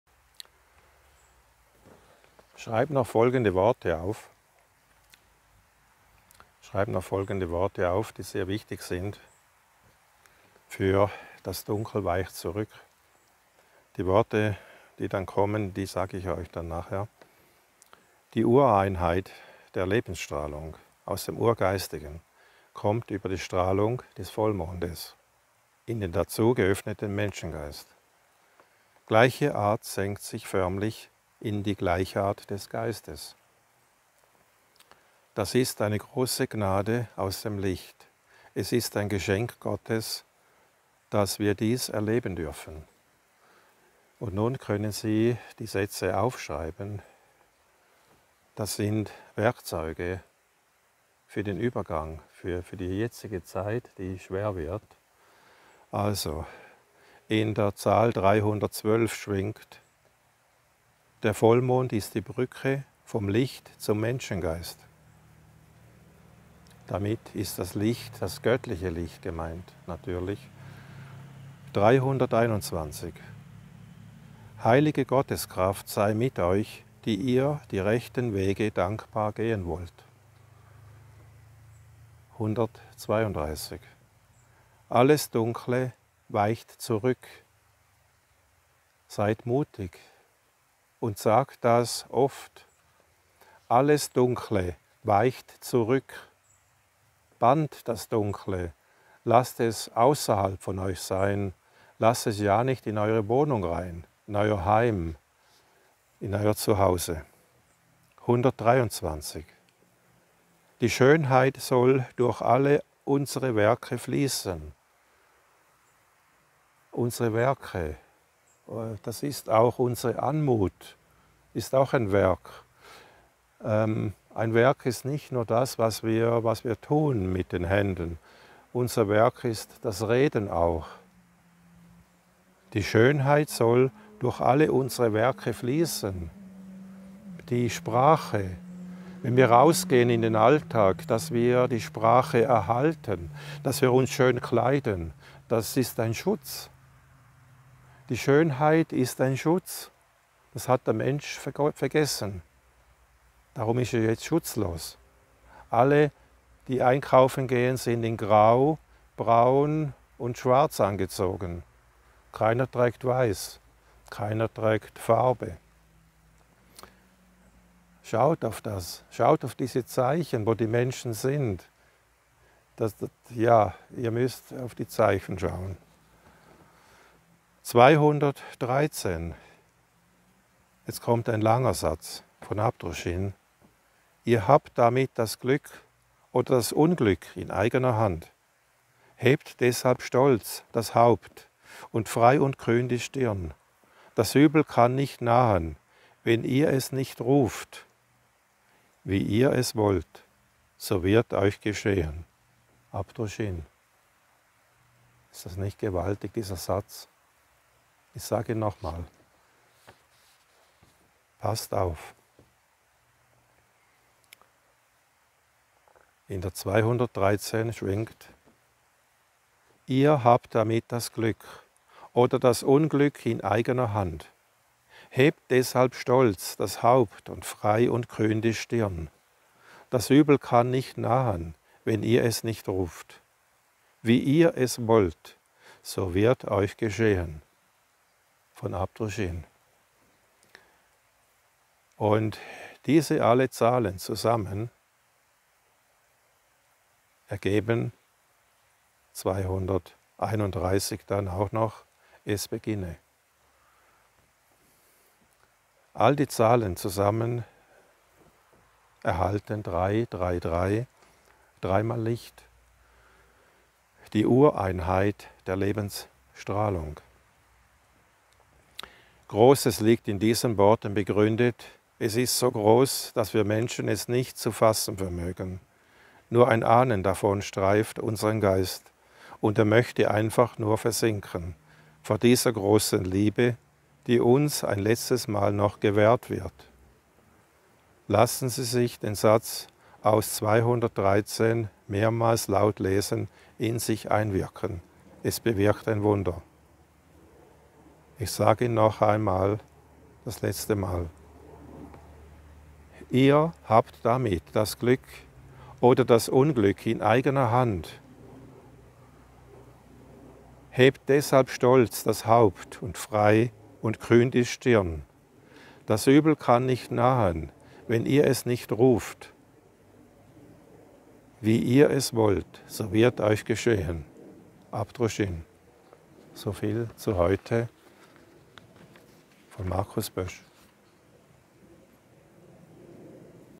Helfende Worte in dem Chaos des Corona, von Markus Bösch, zum Aufschreiben.
Die Botschaft von Abdrushin "Im Lichte der Wahrheit" gibt dem Leser eine neue Ausrichtung für sein weiteres Leben, als Stütze und Führung durch allen Chaos und Wirren der heutigen Zeit, die Botschaft ist ein Stab auf dem Weg zum Licht!